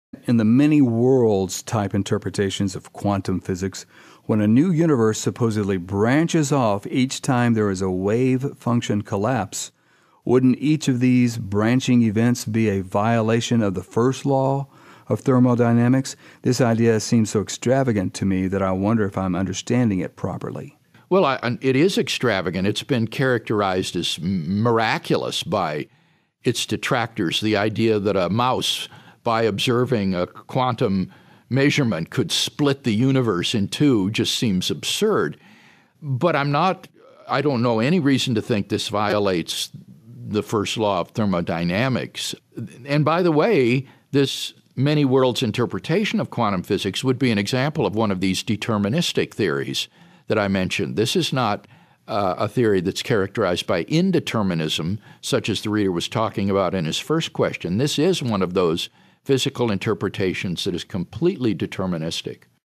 Is The Many Worlds' branching of Universes a Violation of the First law of Thermodynamics?
- This question was influenced by Victor Stenger: Is The Many Worlds' branching of Universes a Violation of the First law of Thermodynamics. Dr. William Lane Craig answers. This is part of the "Defending the Kalam Cosmological Argument" series